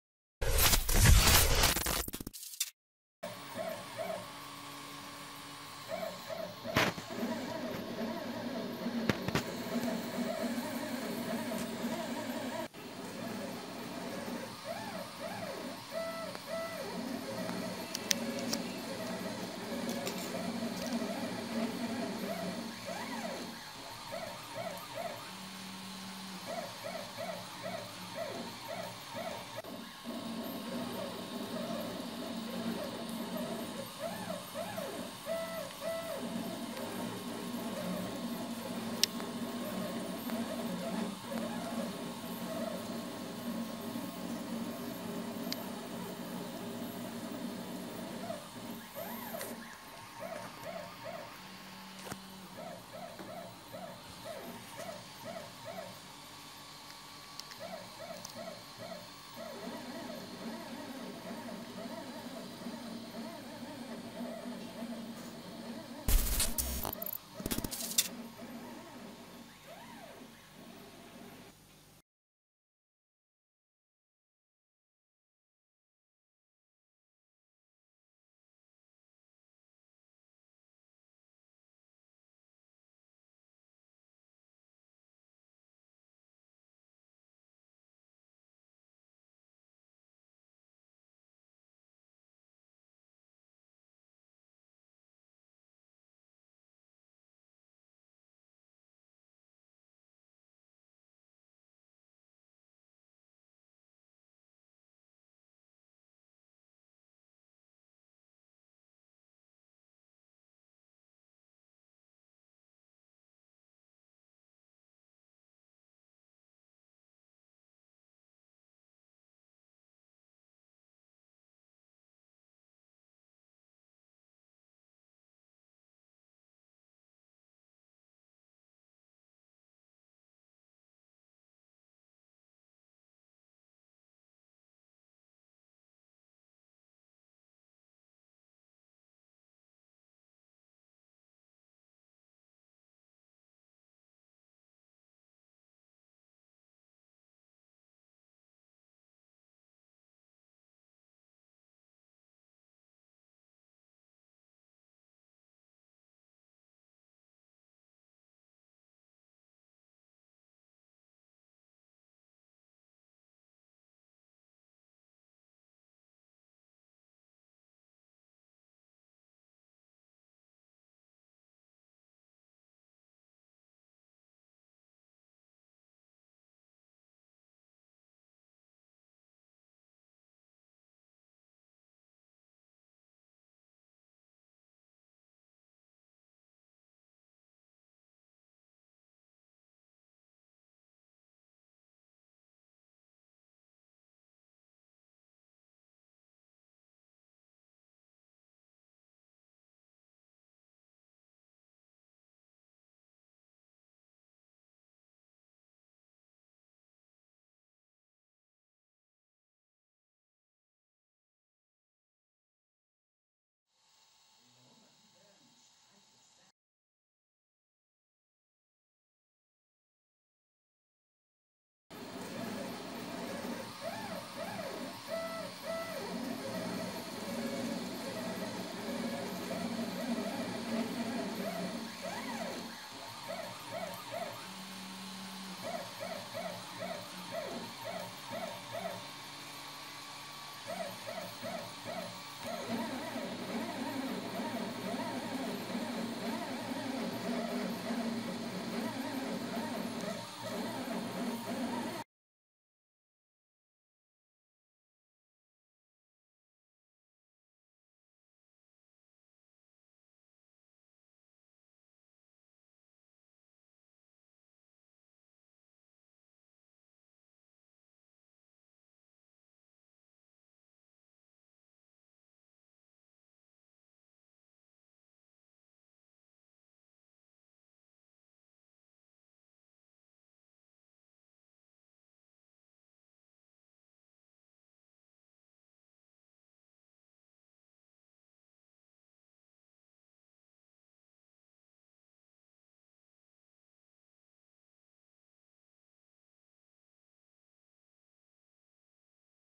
3D Printing - Mounting System for smartphone
This project is mounting system for phones , can used to make movies and take time lapse video .